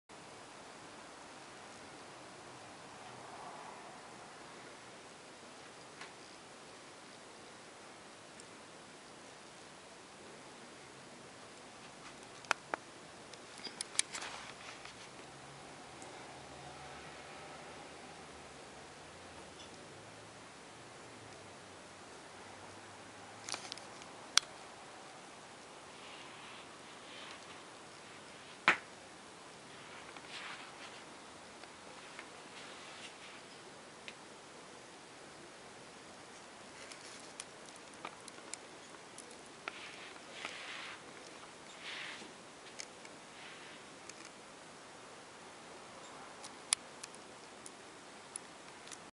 Cat vs Animal
This is what my cat does when no one is home